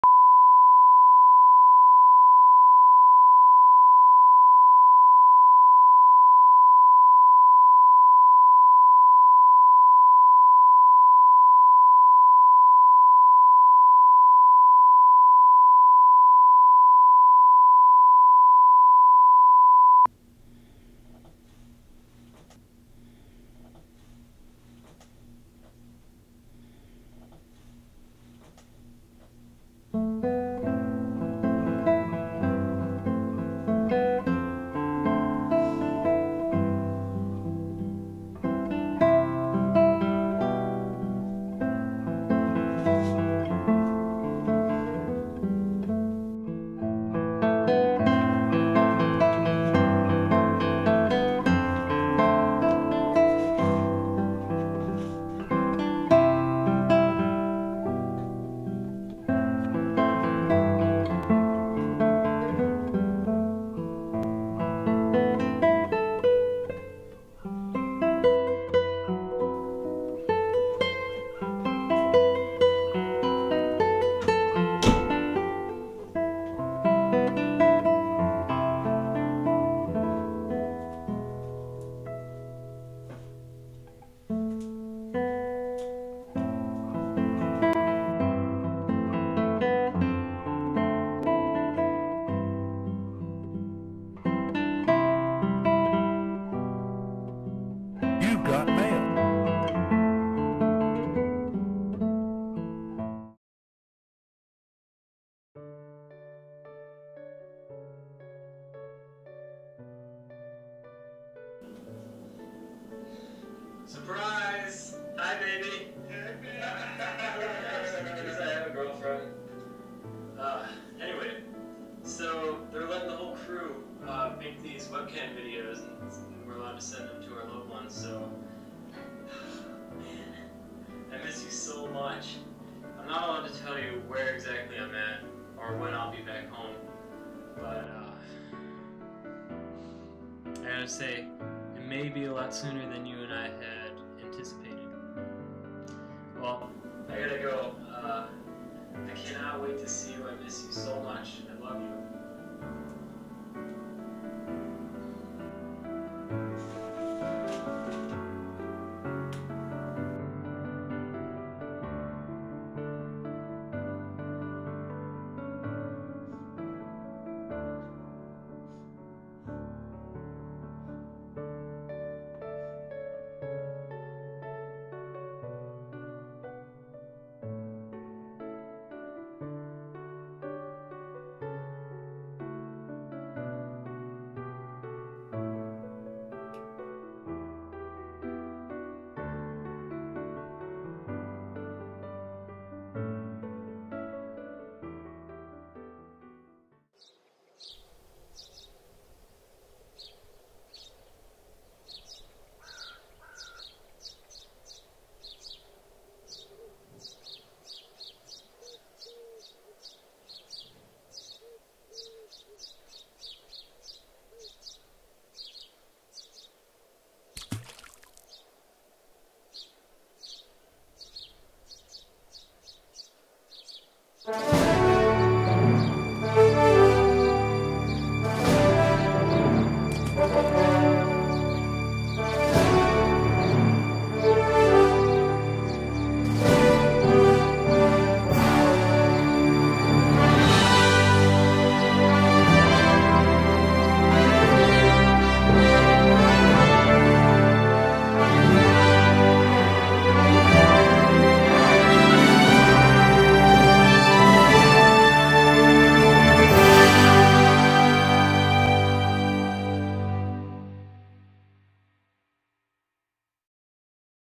The Match Action
A short cheesy little naval story that I made for my editing class at The New England Institute of Art. May not be the best quality, but it's a story.